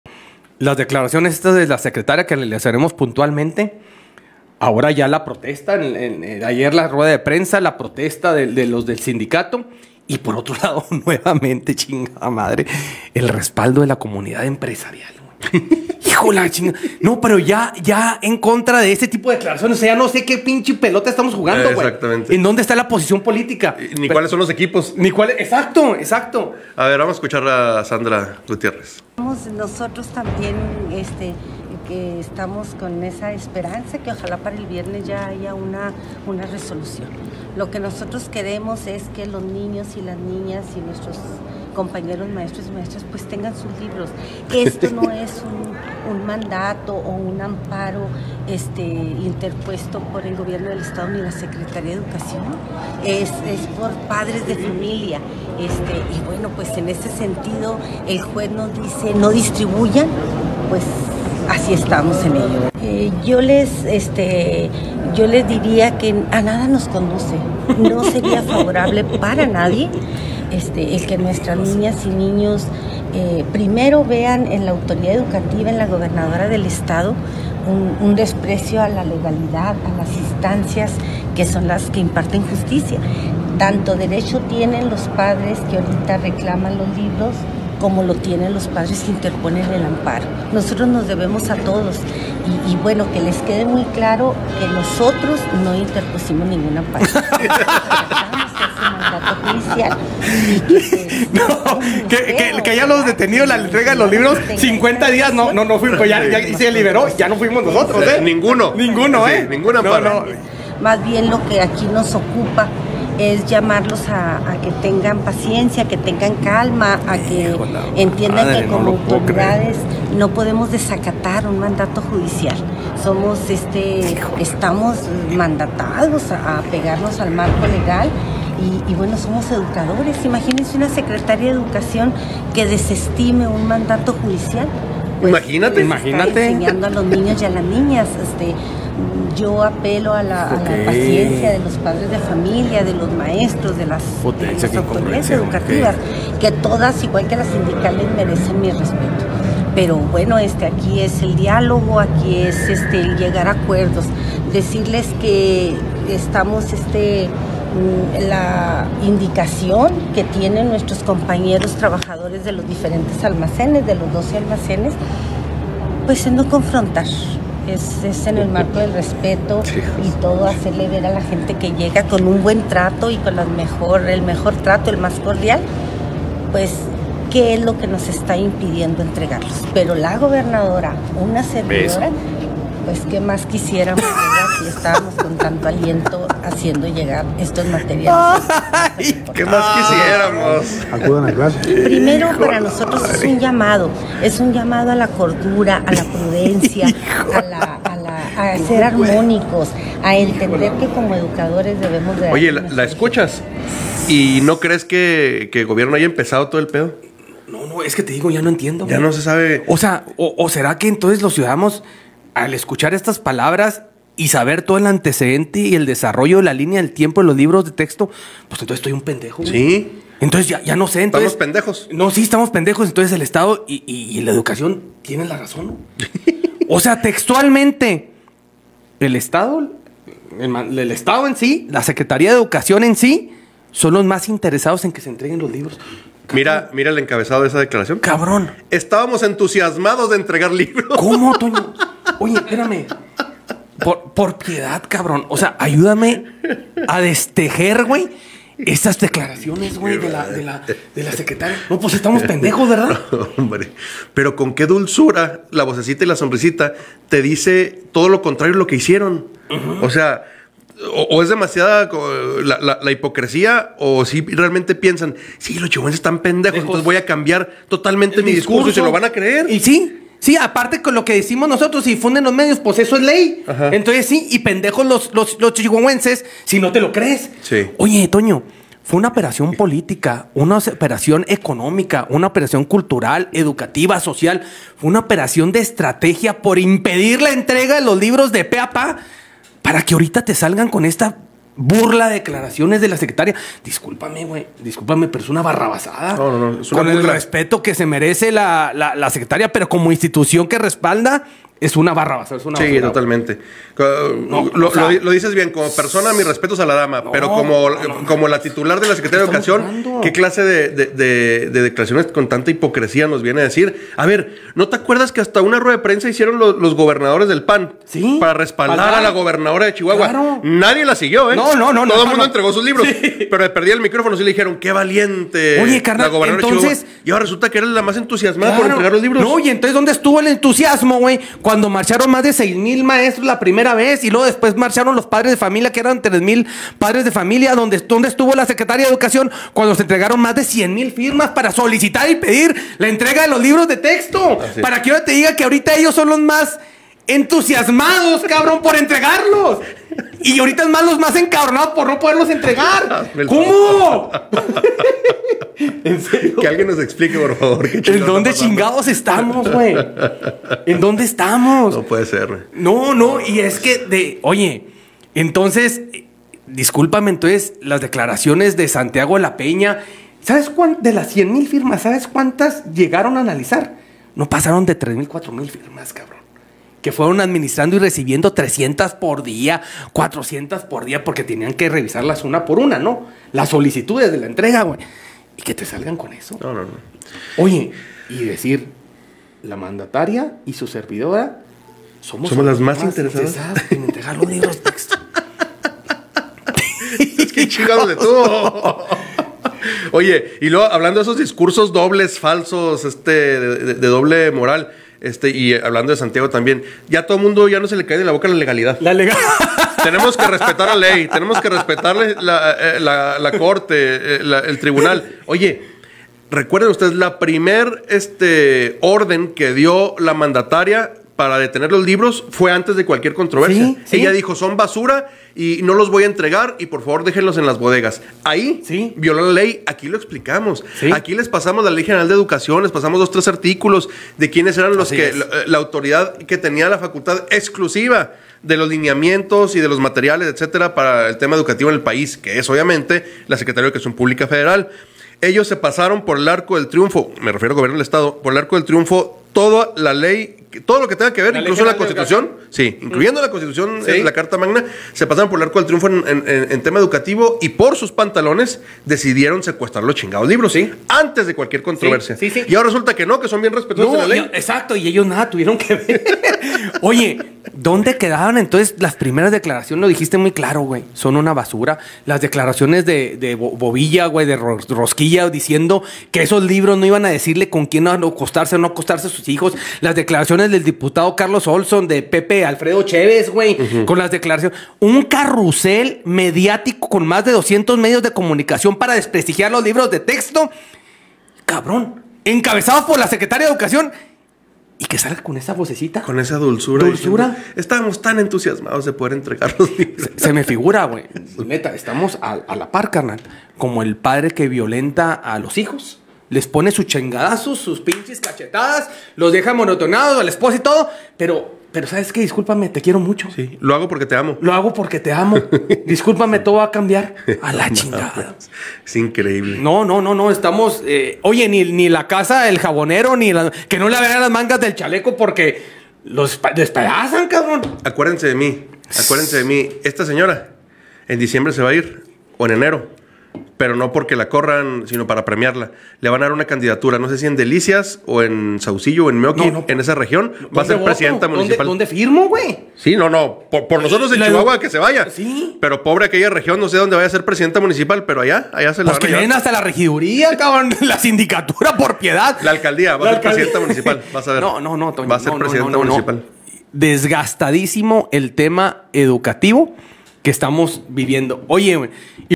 Aqui un claro ejemplo de lo que significa "hipocresía"
Aquí analizamos las increíbles palabras de la Secretaria de Educación Sandra Gutiérrez, para que usted que amablemente nos hace el favor de seguirnos, decida si quiere reír, llorar, enojarse o hasta maravillarse. Ahora resulta que ella y tooodo Gobierno del Estado estaban muy entusiasmados con repartir los libros, que les puede mucho que el reparto esté suspendido por un amparo y que de verdad desean entregarlos lo más pronto posible. Definitivamente hay gobernantes que realmente piensan que sus gobernados están pendejos y que cualquier cosa que digan se las van a creer; aquí un claro ejemplo de ello...